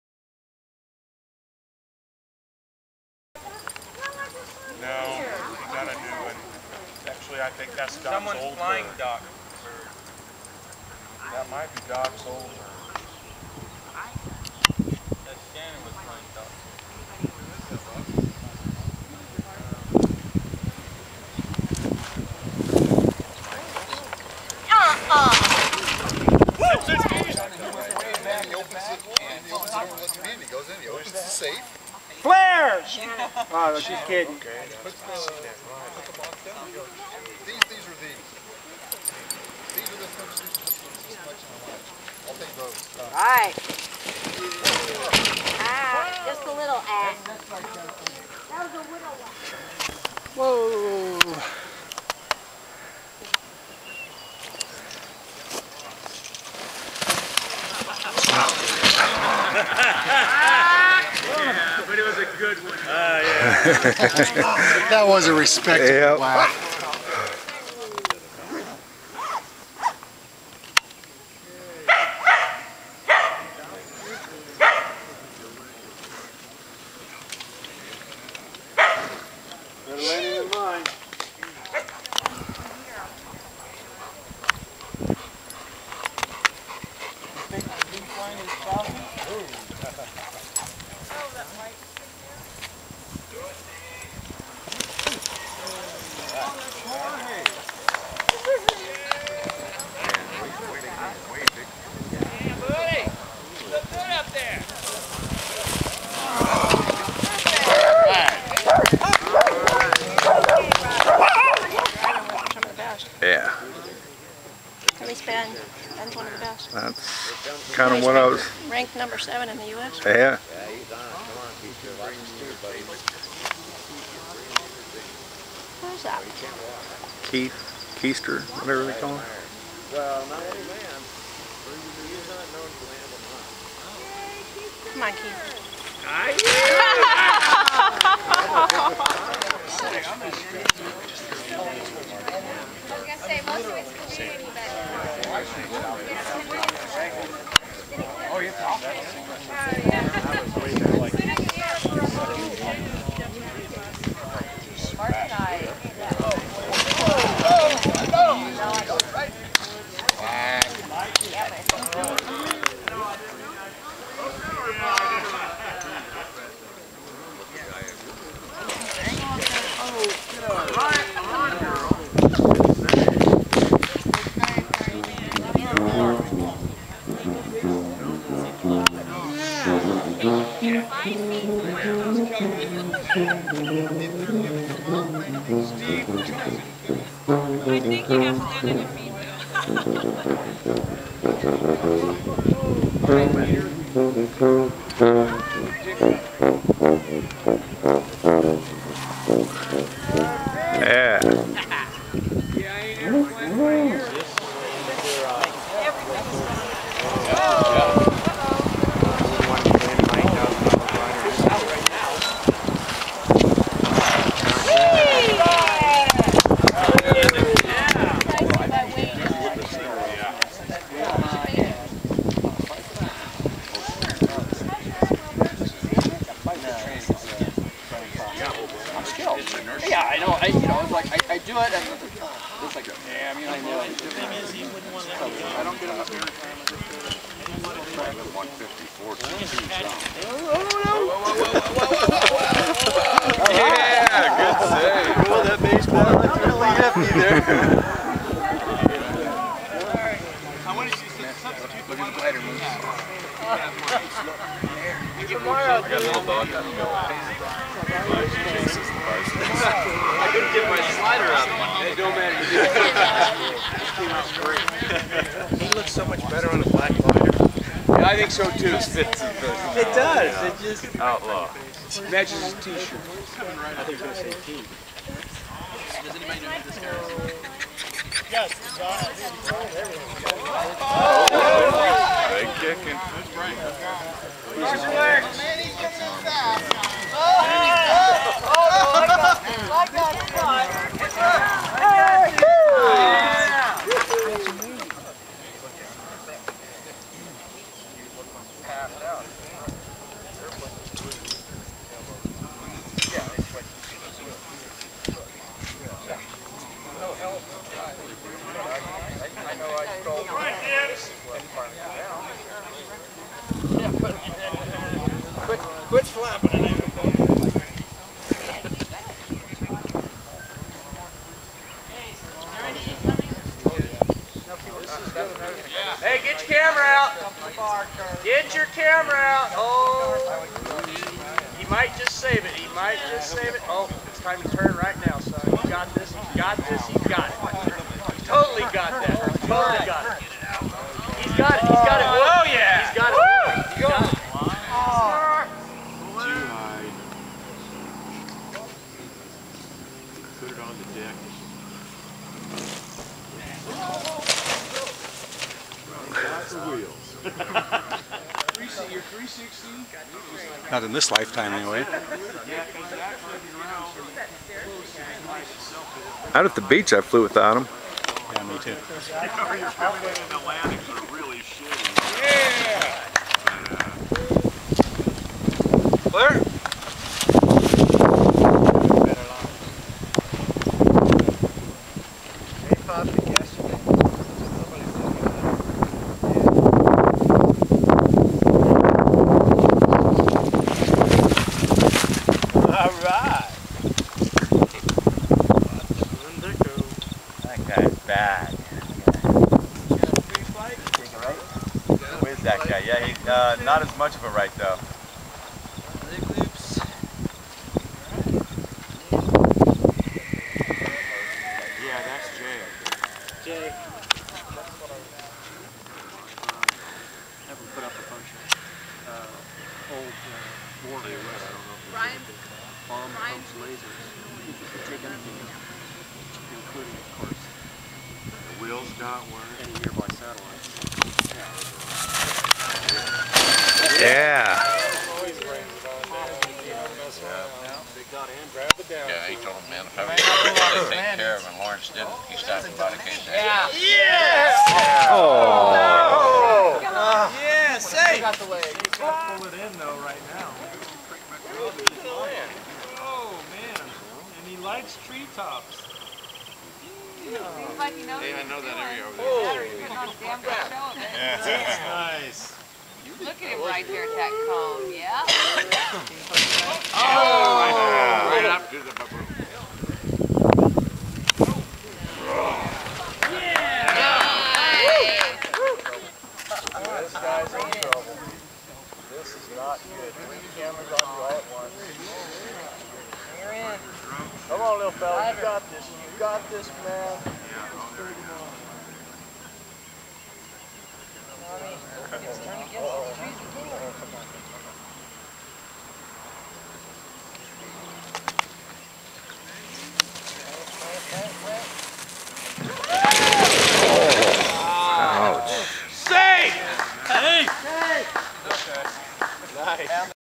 Cyote Howl 2010.wmv
People were great, flying was good, my landings sucked.
Nice to be the one pointing the camera.